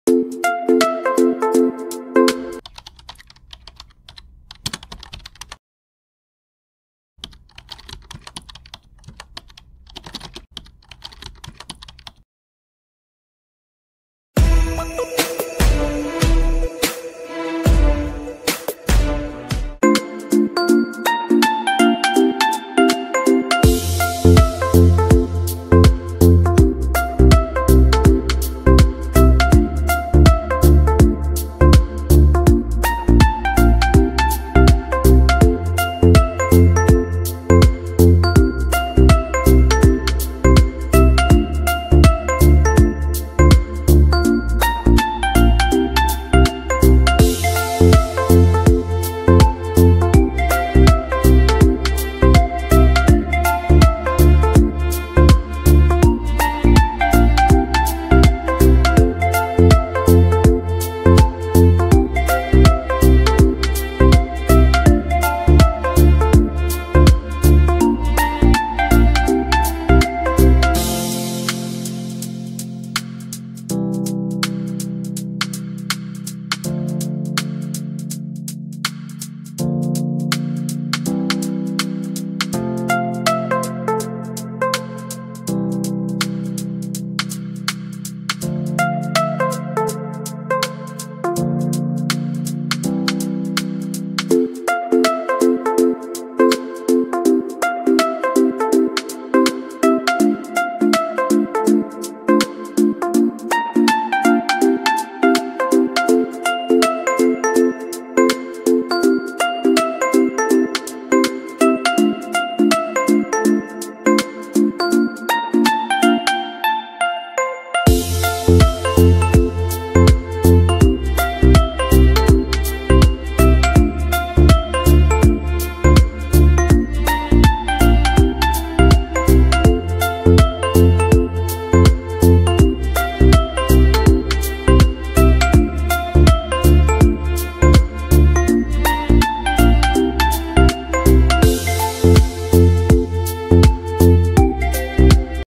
IPhone Stuck on Red Charger: Diy Fix how
I just want to share how I fixed my iPhone e 5c stuck on Red charger